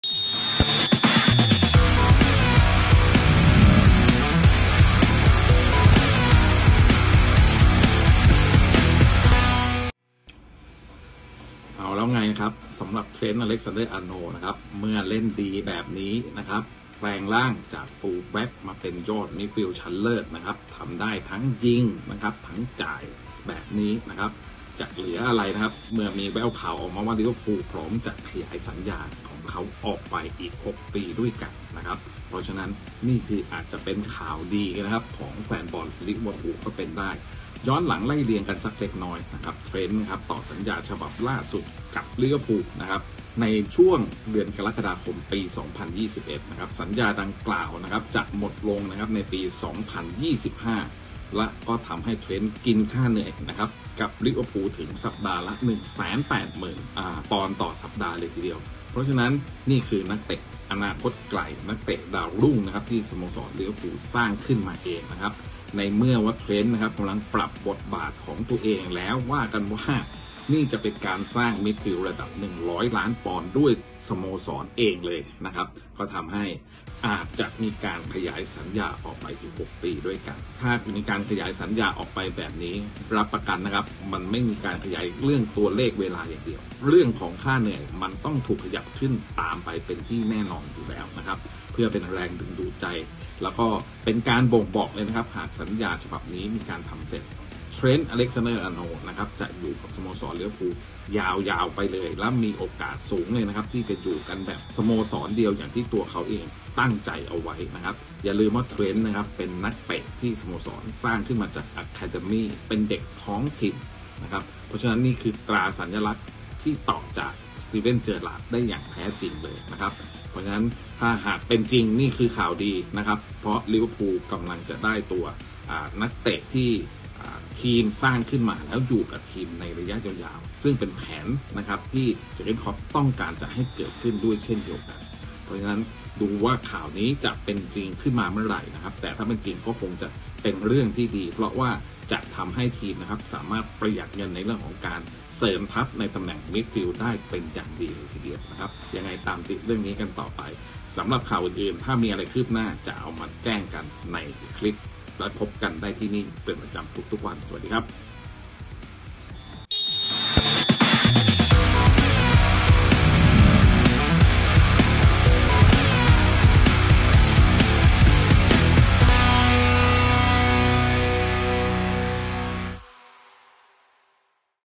หงส์หวงเตรียมต่อสัญญาเทรนท์เพิ่ม 6 ปี
หลังจากฟอร์มดี ลิเวอร์พูลไม่รอช้าพร้อมประเคนสัญญาใหม่ให้เทรนท์ยาวๆ